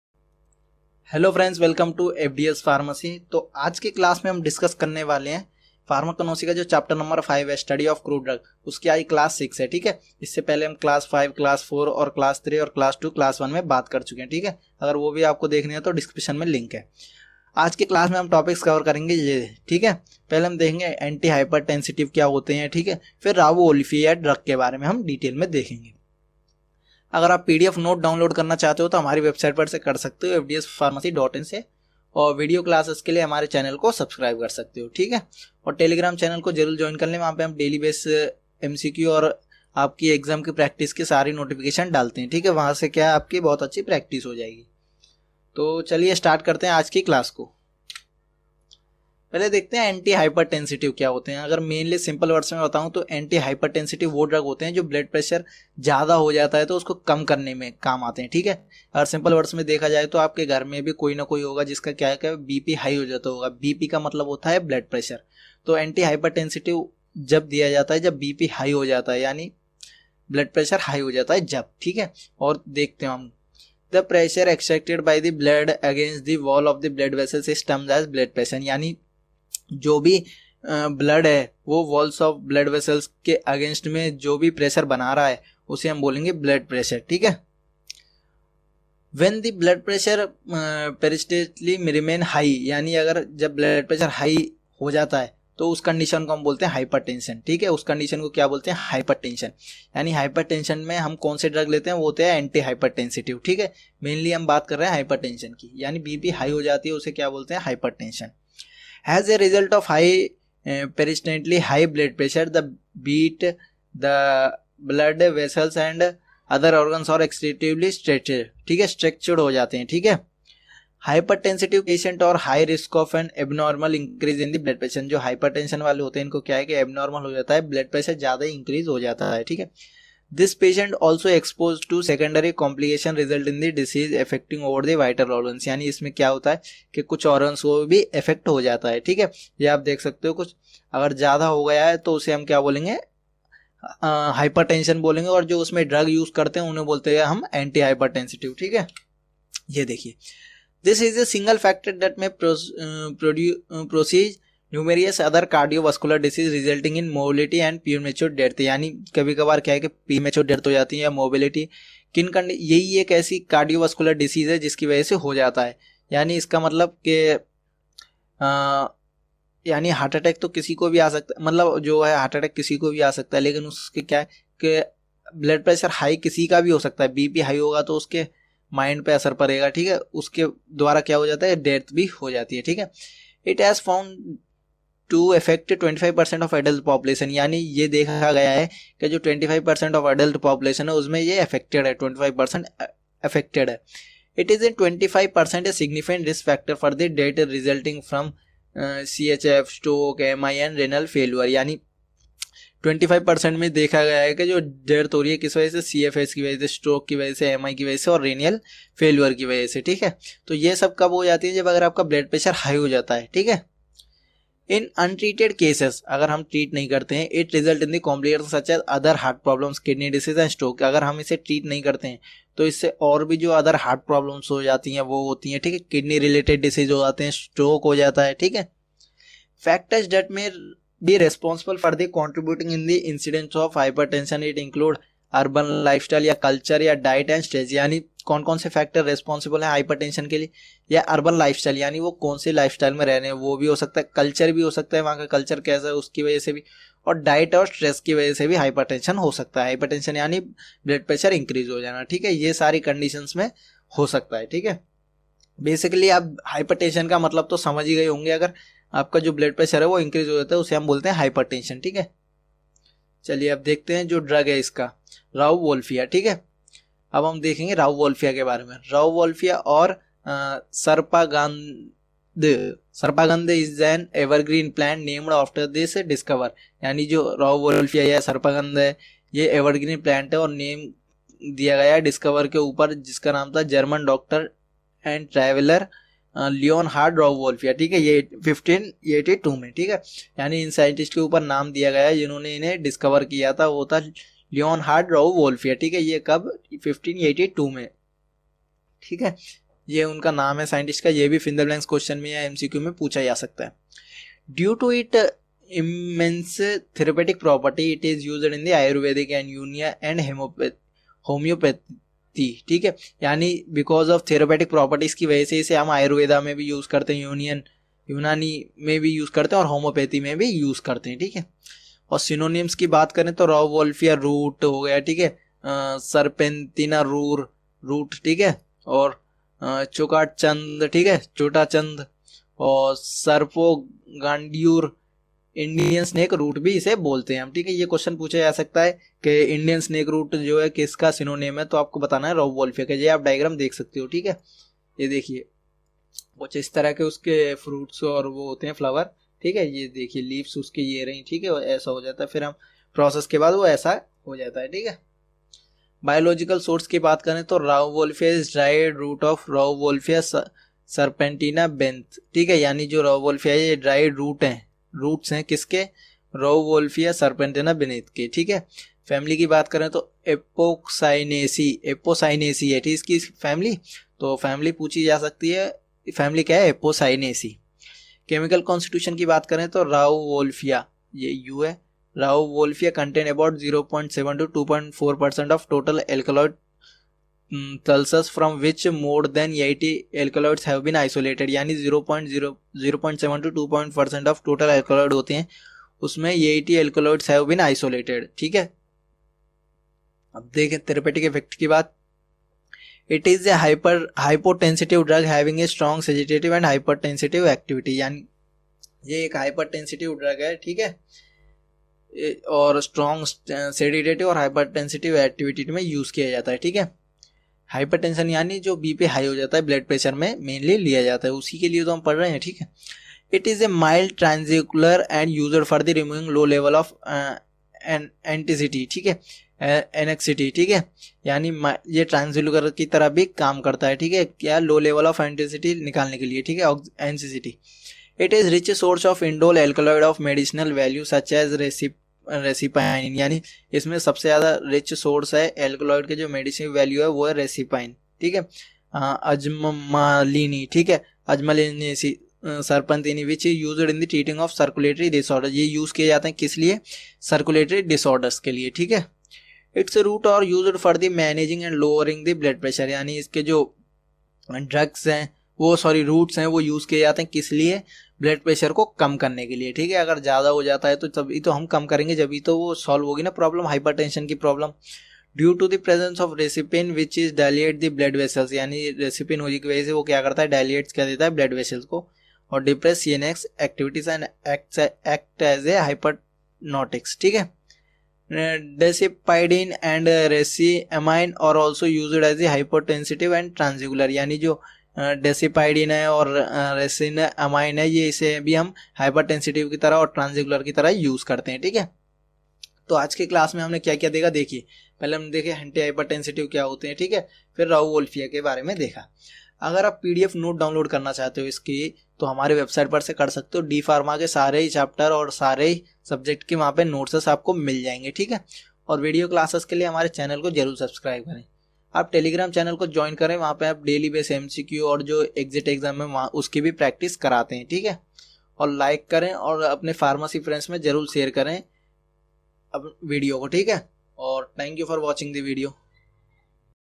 Anti - Hypertensive | L-6 | Study of Crude Drugs : CH-5 | Pharmacognosy | D.Pharma 1st Year Notes
Diploma in Pharmacy [ As Per New Syllabus ER-20 ]
Pharmacognosy { ER20-13T }
Chapter 5 :-  Study of Crude Drugs
Lecture 6 :-  Anti - Hypertensive
[ Download FREE PDF Notes ]

Topics 👇
00:00:00 ➡ Anti - Hypertensive
00:04:25 ➡ Rauwolfia

KEEP SUPPORTING🤕